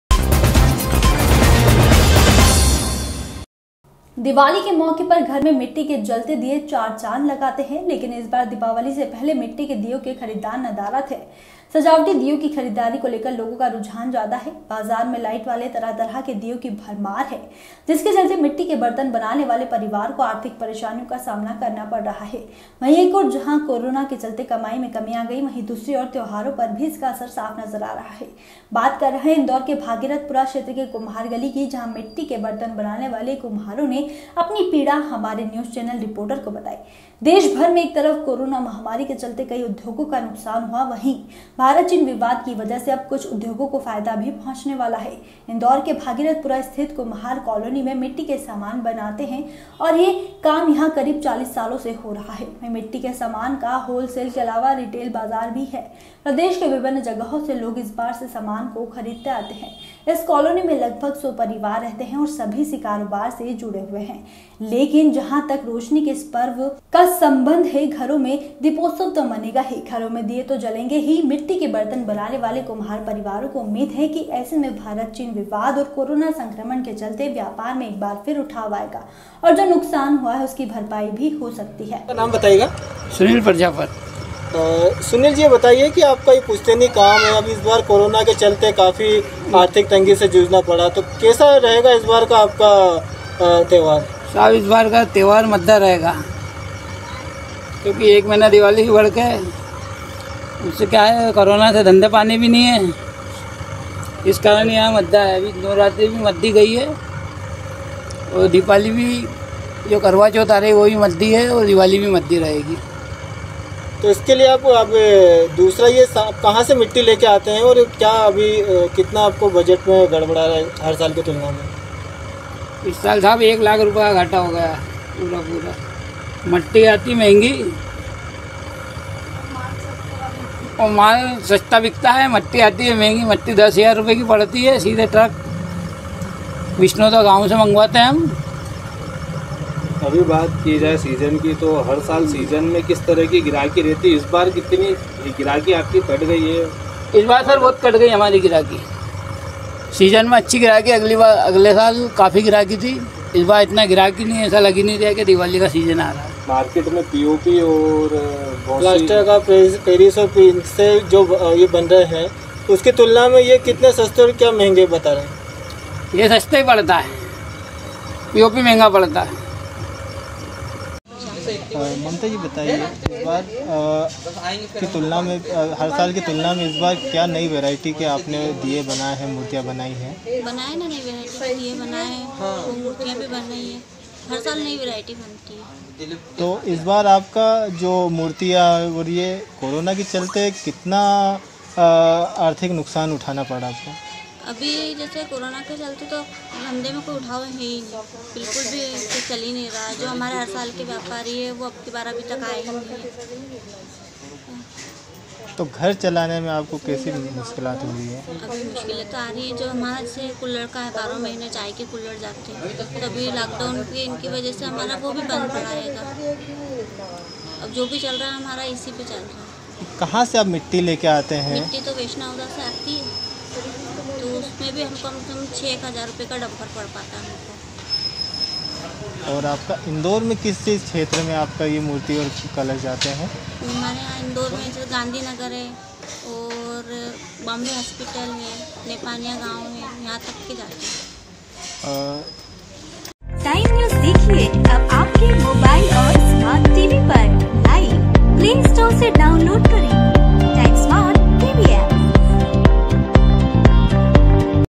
दीपावली पर्व पर बिकने वाले मिट्टी के दियो पर कोरोना का भरी मात्रा में हुवा असर
दिवाली के मौके पर घर में मिट्टी के जलते दीये चार चांद लगाते हैं। लेकिन, इस बार दिवाली से पहले मिट्टी के दीयों के खरीदार नदारद हैं। सजावटी दीयों की खरीदारी को लेकर लोगों का रुझान ज्यादा है। बाजार में लाइट वाले तरह-तरह के दीयों की भरमार है। जिसके चलते मिट्टी के बर्तन बनाने वाले परिवार को आर्थिक परेशानियों का सामना करना पड़ रहा है वही एक और जहां कोरोना के चलते कमाई में कमी आ गई है वहीं दूसरी ओर त्योहारों पर भी इसका असर साफ नजर आ रहा है बात कर रहे हैं इंदौर के भागीरथपुरा क्षेत्र के कुम्हार गली की जहां मिट्टी के बर्तन बनाने वाले कुम्हारों ने अपनी पीड़ा हमारे न्यूज़ चैनल रिपोर्टर को बताइए।